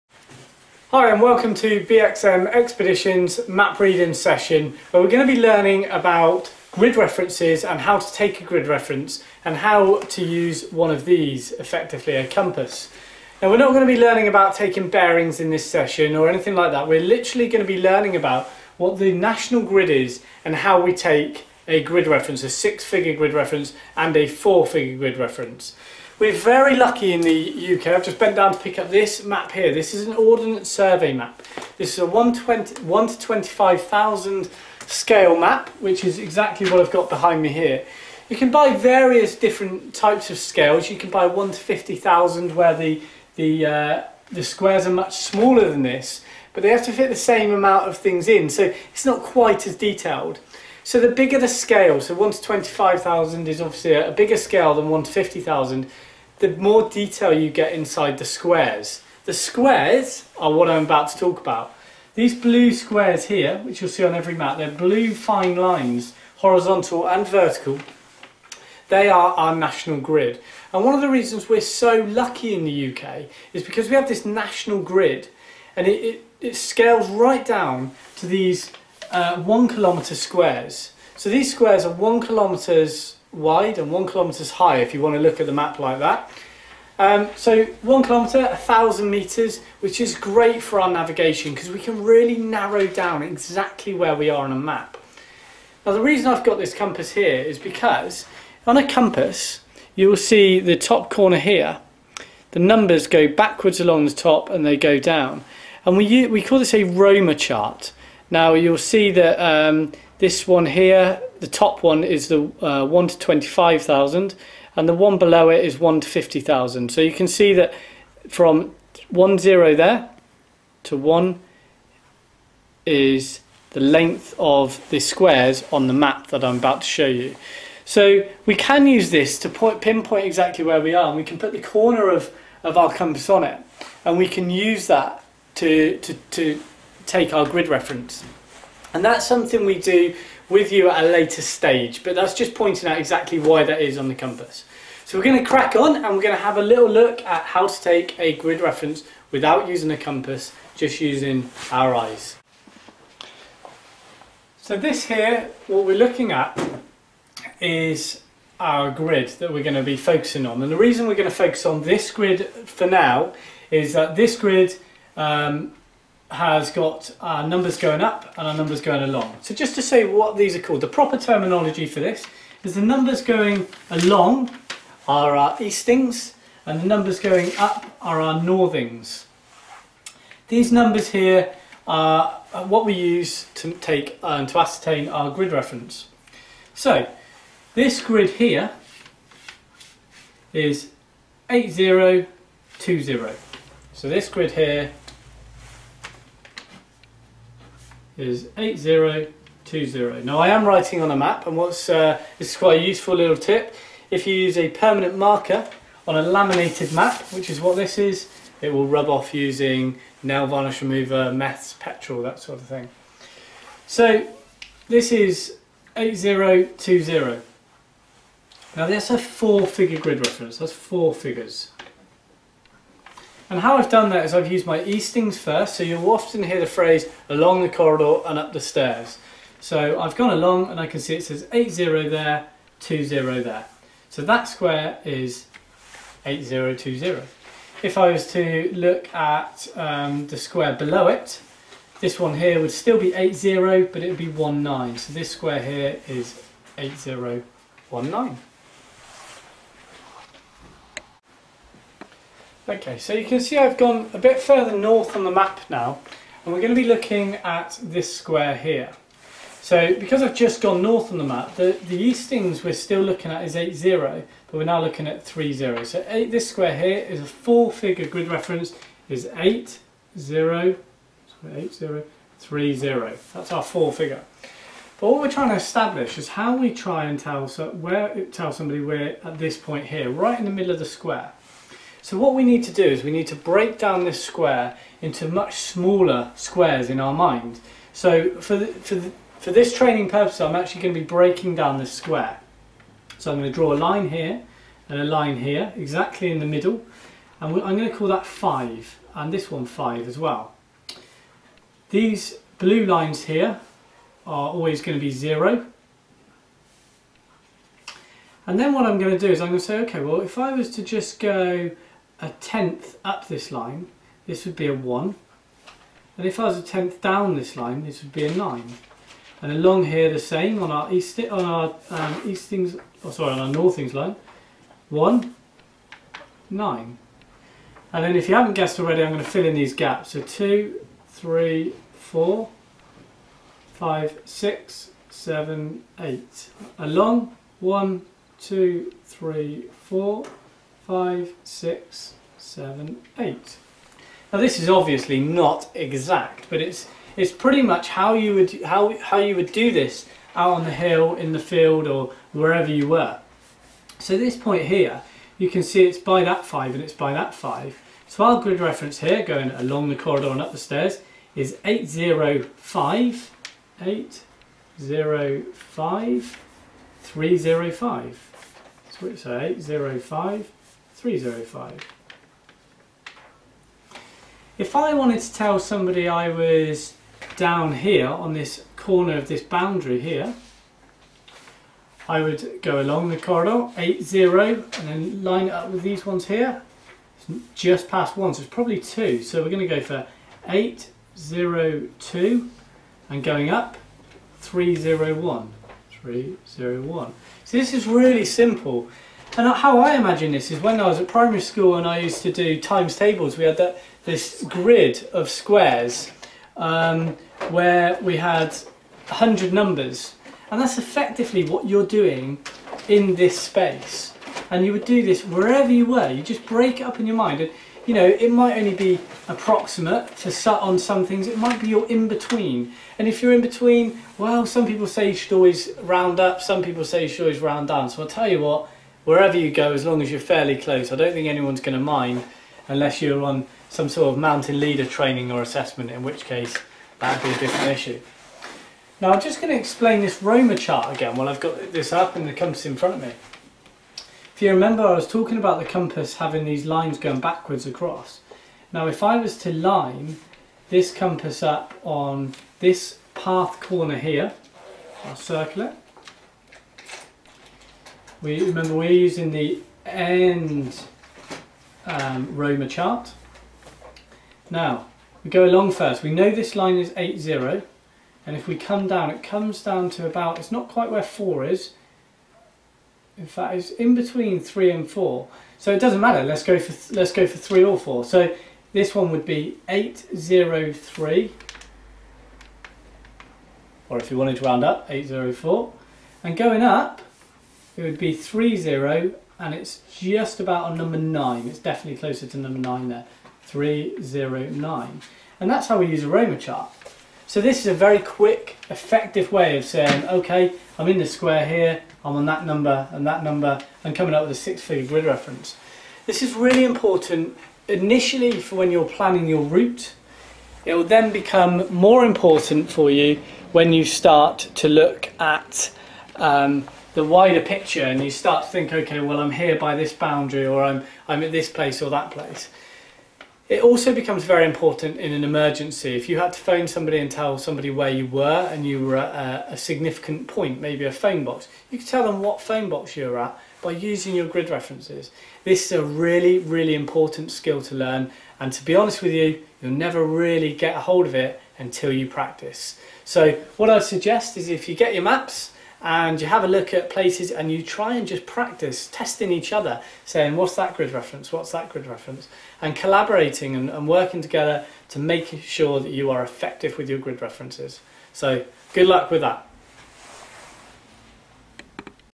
Grid References, Map Reading, Duke of Edinburgh's Award, Training, Schools
Ben, the director of BXM Expeditions shows how to take both 4 and 6 figure Grid references on a map. Great for DofE, Duke of Edinburgh's award participants or schools to use.